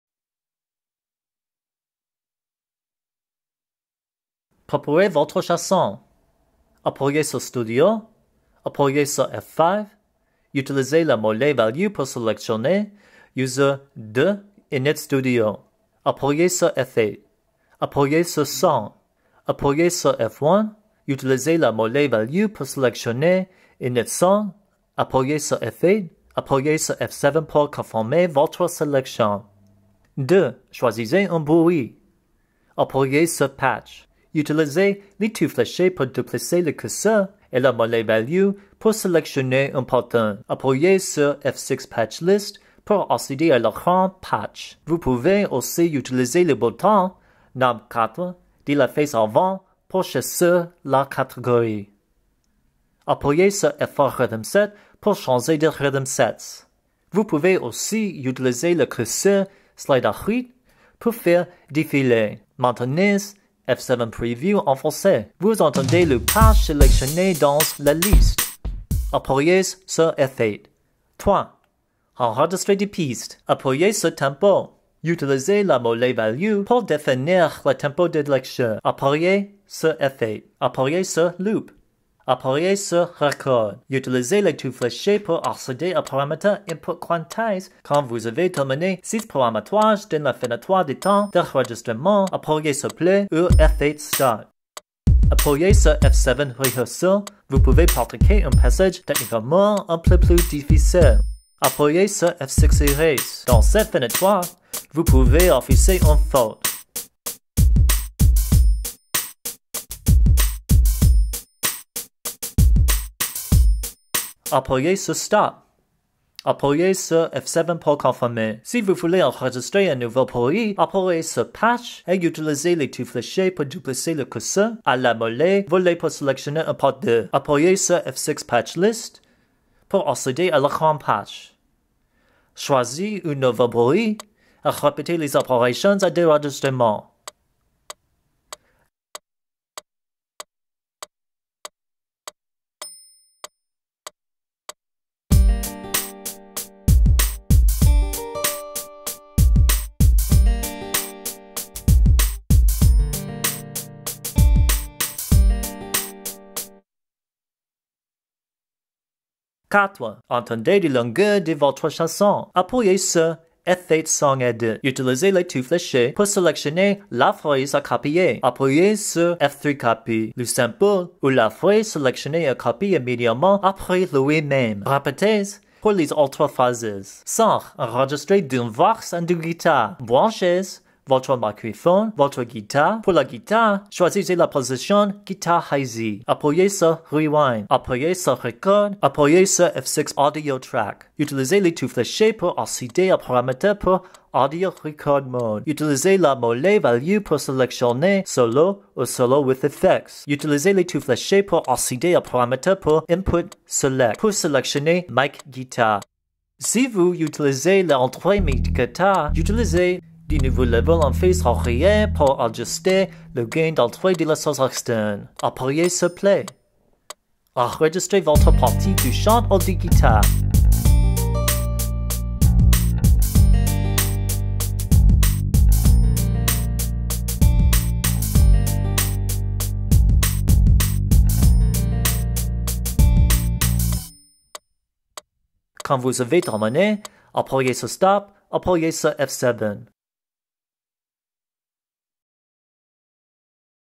Comment fait-on un enregistrement dans le Fantom-G?
Je décris dans cette video.  Vous apprenez enregistrement dans le Fanton-G.